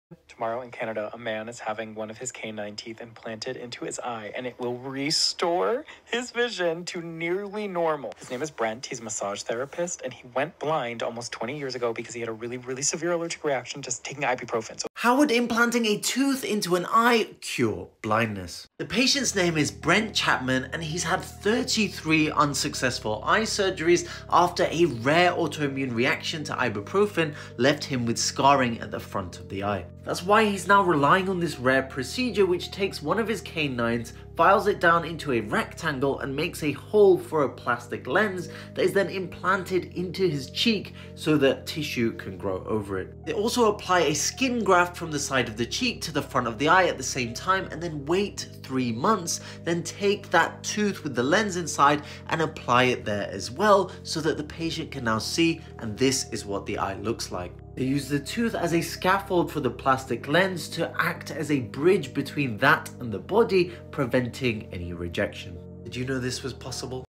“They’re Reversing Blindness With a Human Tooth”
A tooth implant in the eye might sound unusual, but it’s actually part of a groundbreaking procedure called osteo-odonto-keratoprosthesis (OOKP), which can restore sight to people with severe corneal damage. This technique involves extracting a tooth, shaping it into a small frame, and implanting an artificial lens into it. The modified tooth is then placed into the patient’s cheek for several months, allowing it to develop a blood supply. Once it’s ready, the tooth-lens structure is surgically implanted into the eye, where it functions as an artificial cornea, allowing light to pass through and restoring vision.

This method is particularly effective for patients whose eyes cannot accept traditional corneal transplants, such as those with severe scarring, burns, or autoimmune conditions that have damaged the cornea. The reason a tooth is used is that the body is less likely to reject its own tissue, making the implant more stable. While the procedure is complex and reserved for extreme cases, it has successfully restored vision in patients who were previously considered permanently blind, offering hope to those with few other options.